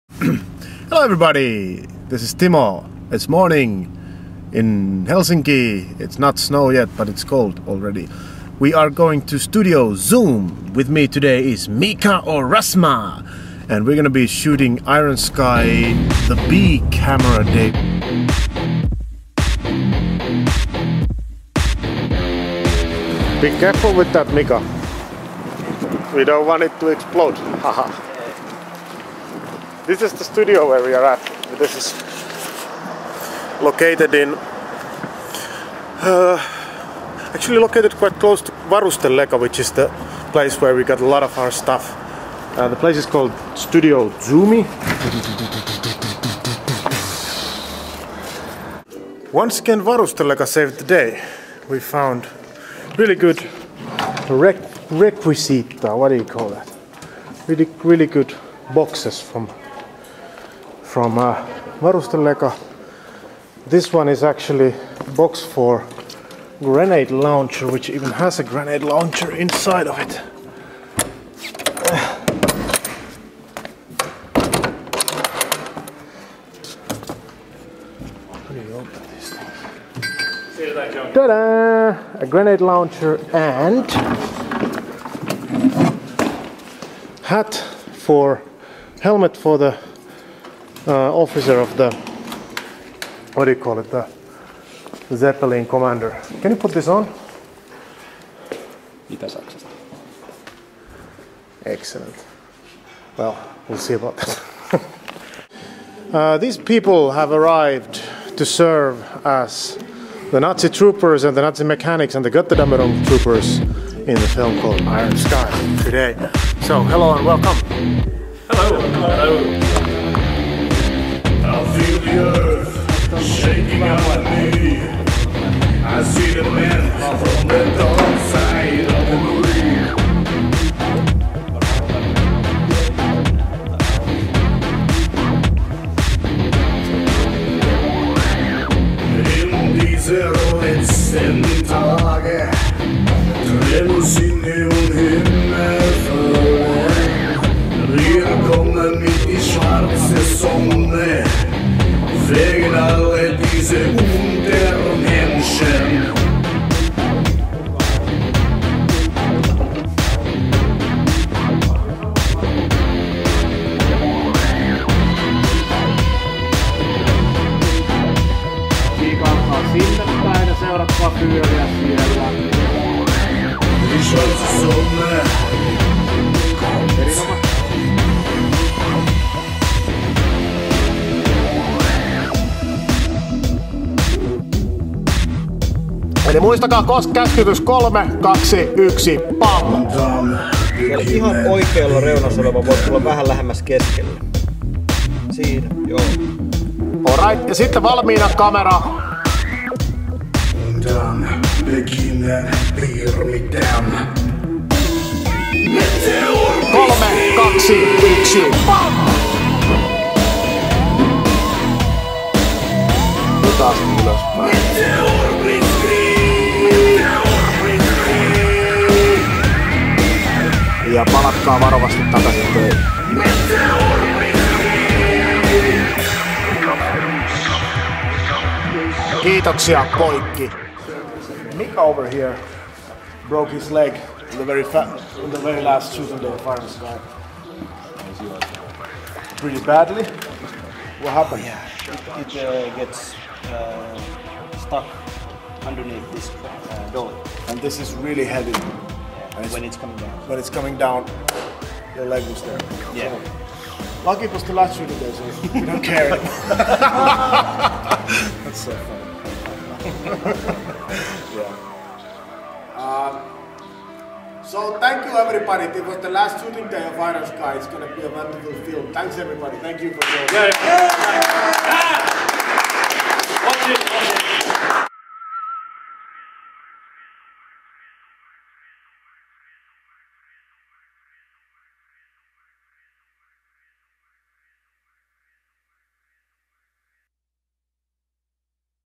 Director's Diary: B-Cam Day!
The last time we shot anything for Iron Sky took place in Helsinki, Finland, where we shot some background characters to populate the Moon Base.

Music was provided this time by Herr Luger, who was one of the Extras, and got inspired by the day and brought us with this awesome song!